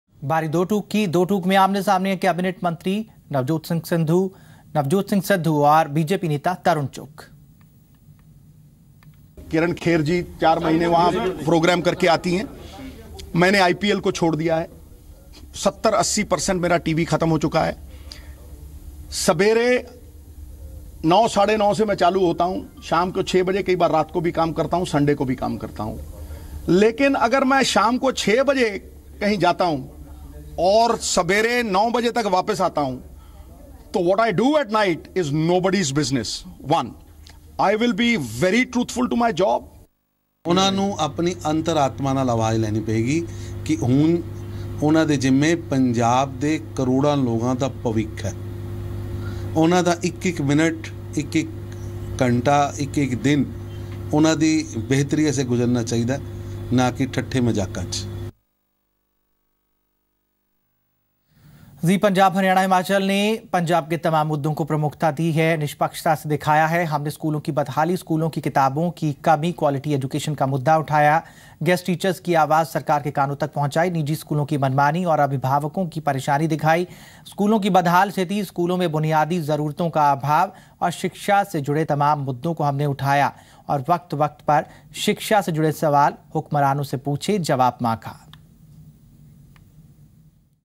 Navjot Singh Sidhu in word war with BJP leader Tarun Chugh on continuing TV show
Navjot Singh Sidhu in word war with BJP leader Tarun Chugh on continuing TV show . Watch complete news story of Haryana Himachal News for getting the detailed news updates!